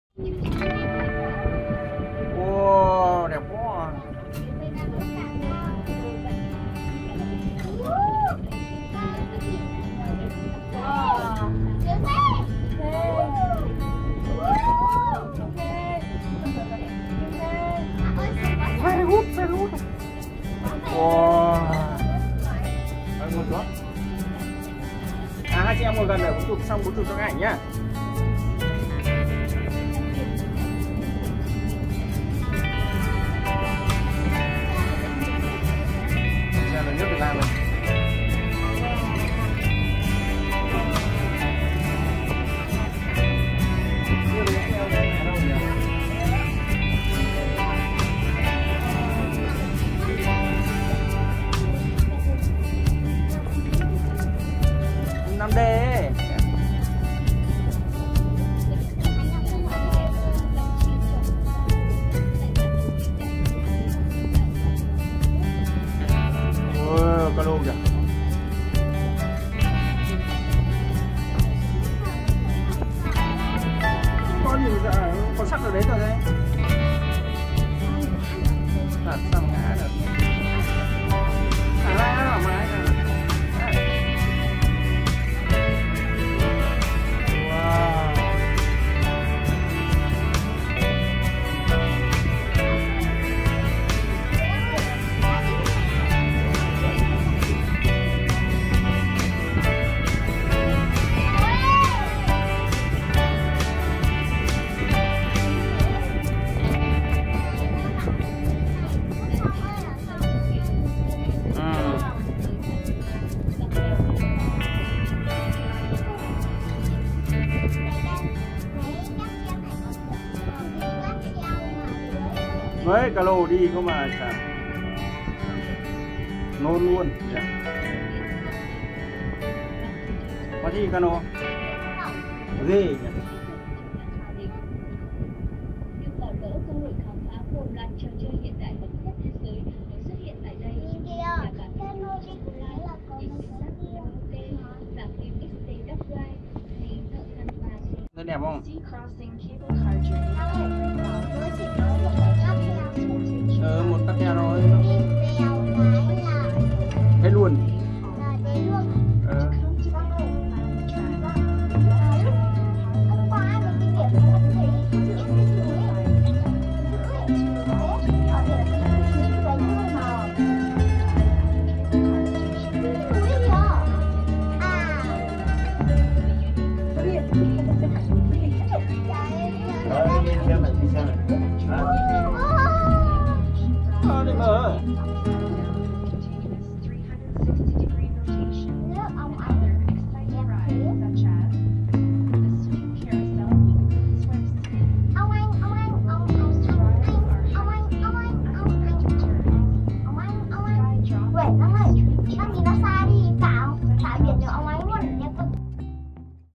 Trải nghiệm cáp treo vượt biển Nha Trang | Hương GIang Vlogs
Cùng Hương GIang Vlog trải nghiệm Hệ thống cáp treo vượt biển Nha Trang
Hệ thống cáp treo Vinpearl Land được thiết kế với 65 cabin loại 8 chỗ với công suất chuyên chở đến 1000 – 1500 khách/giờ. Tuy nhiên, hiện tại cáp treo chỉ đưa vào sử dụng 47 cabin với công suất là 1000 khách/giờ. Với vận tốc di chuyển 6m/s, nên thời gian để đi hết tuyến cáp treo chỉ mất khoảng 12 phút/chuyến.